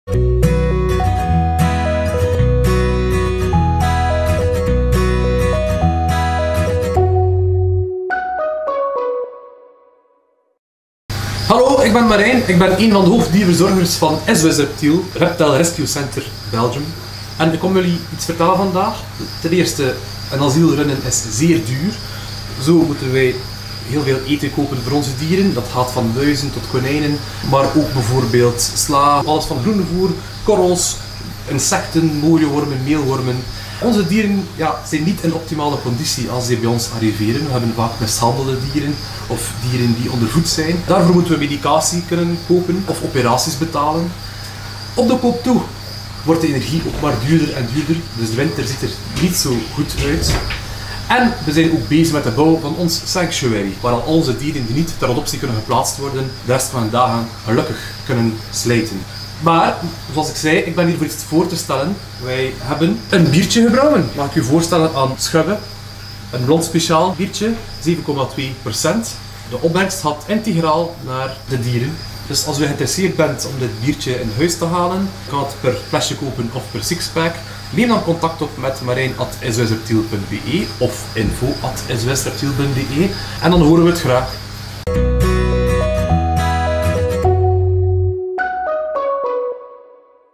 Ontdek Schubbe, ons eigen SOS Reptiel bier!
Sinds kort heeft SOS Reptiel zijn eigen bier: Schubbe. Genoemd naar een van de eerste dieren die door SOS Reptiel werd opgevangen.